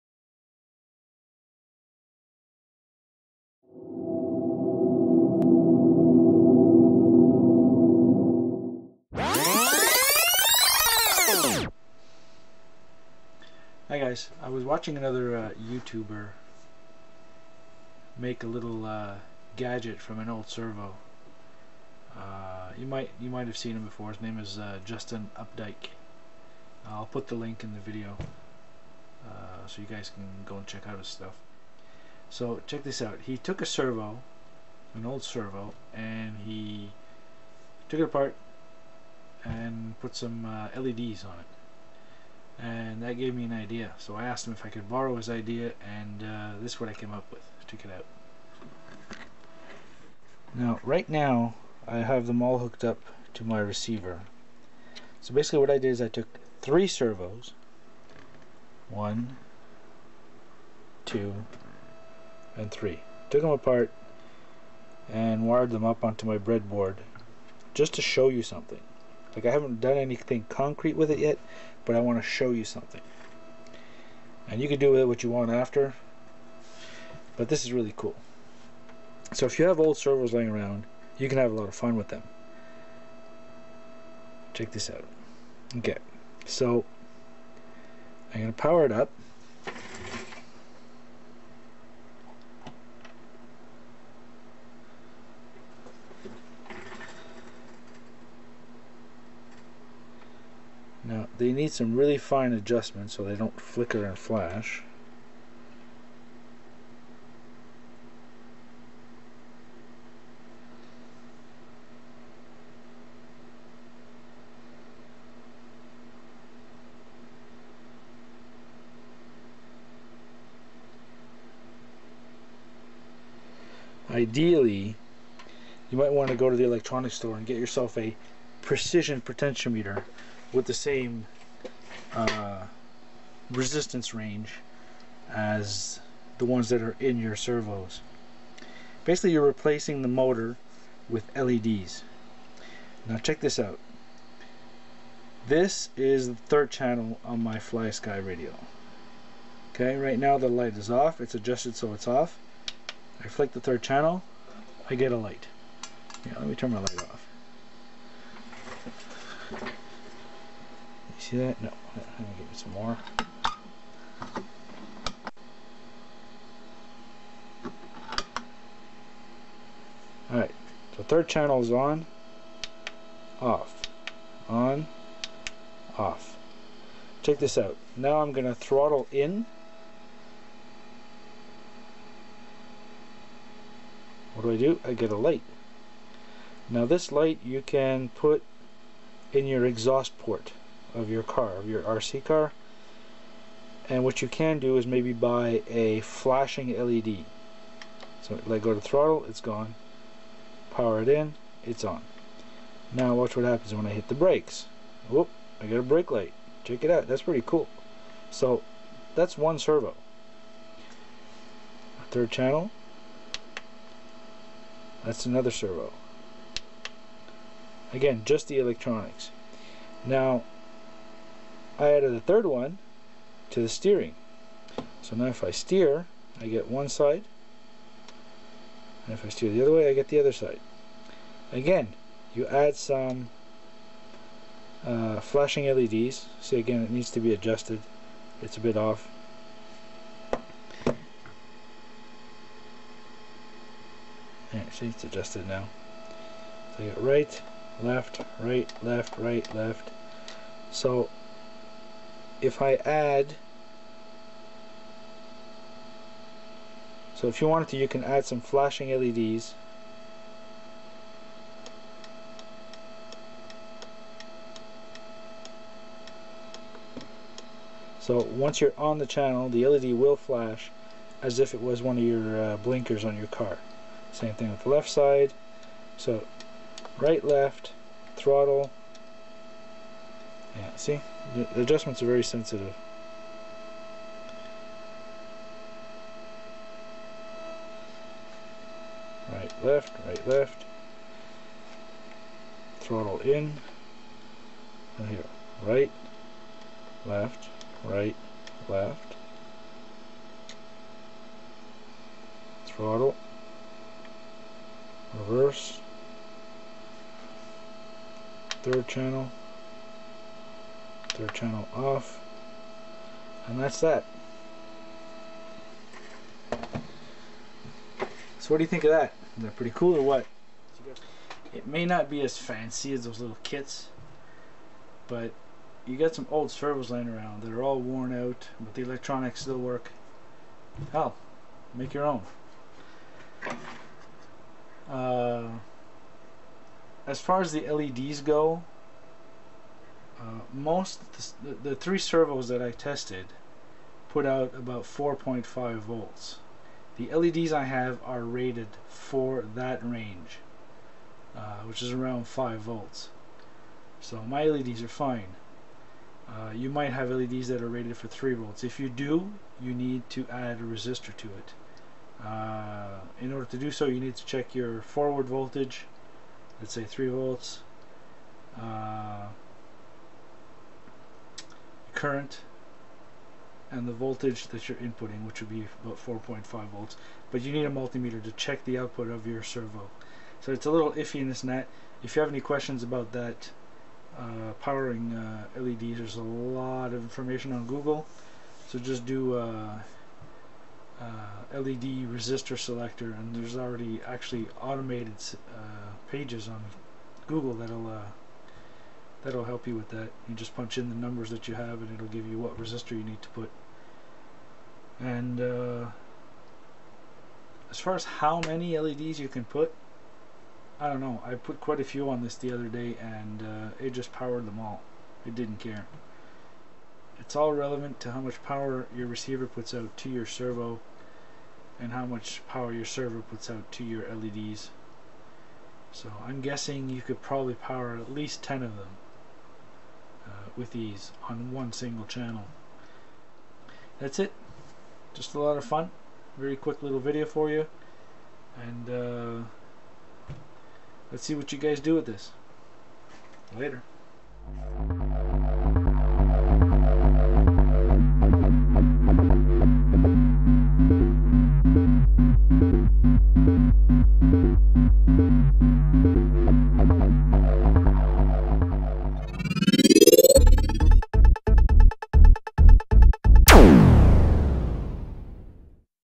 Use old servos to control your RC Car lights. Brakes, gas, 3rd channel.
Do you have some old servos laying around that you might have been thinking you want to toss in the trash. WELL DON"T, Give them to me..NO just kidding. You can use them to make some cool LED controllers.

Now I am not showing you how to do it, just what you can do with it.
If you want to see how to swap the motor for some LED's then check out this video.

Steph